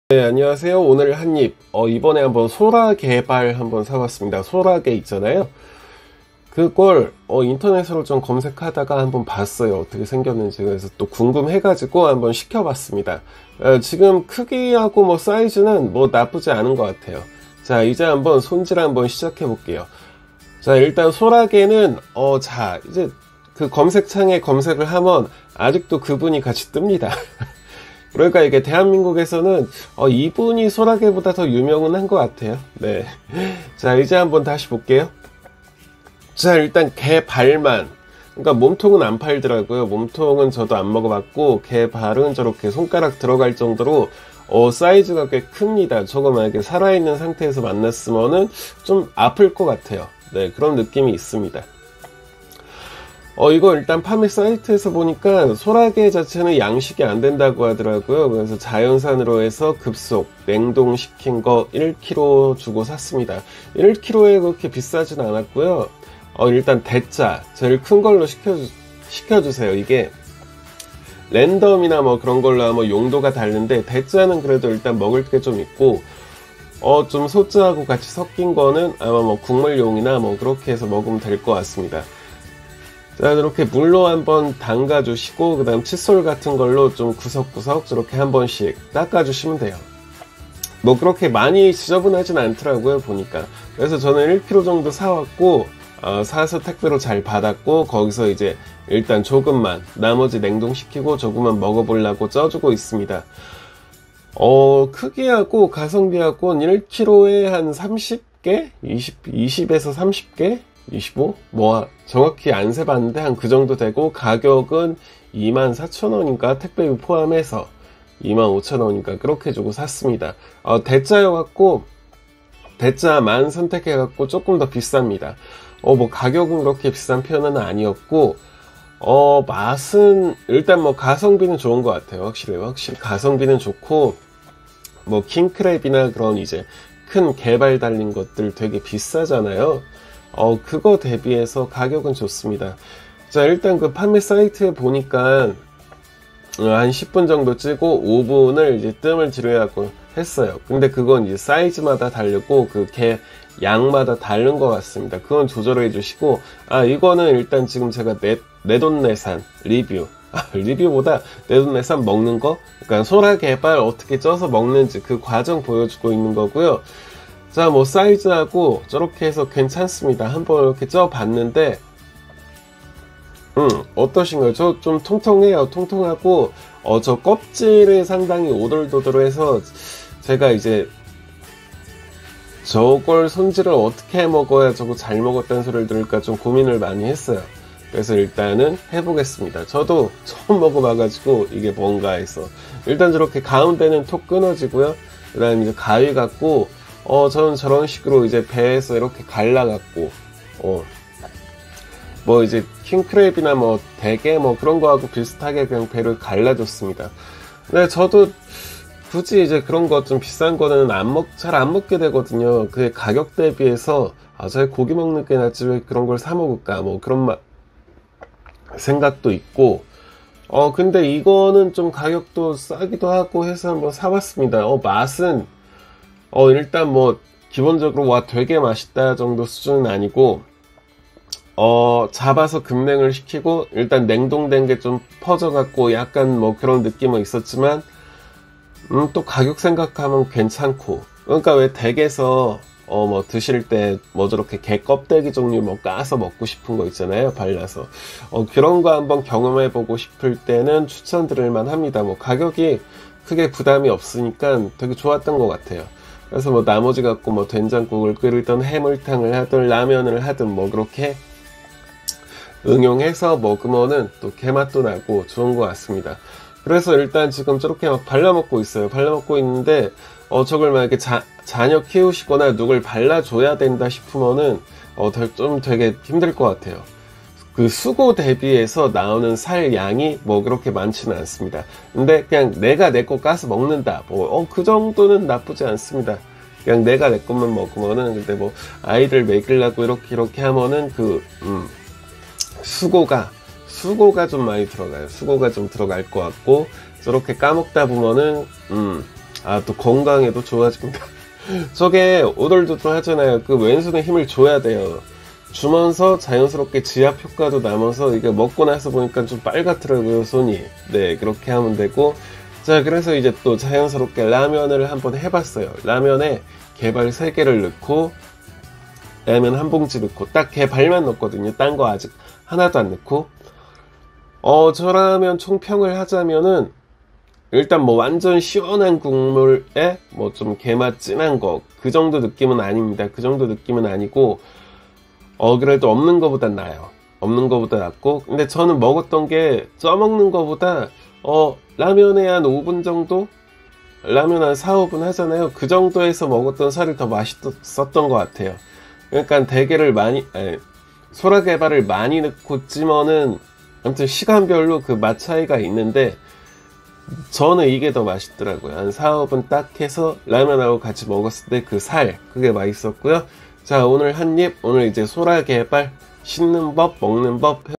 가성비 최강! 대게나 랍스터 말고 이제 이거~ 만원이면 충분합니다! 심심할때 드셔보세요! 소라게발 l 집밥 요리 레시피
안녕하세요~

소라게발 찌는 방법
소라게발라면 입니다.

가성비 좋은 소라게발
살도 많이 있고
좋습니다.

나쁘지 않는 맛에~
저렴한 가격에
푸짐하고~

다용도로 사용이 가능해서 더 좋습니다.

내돈내산 소라게발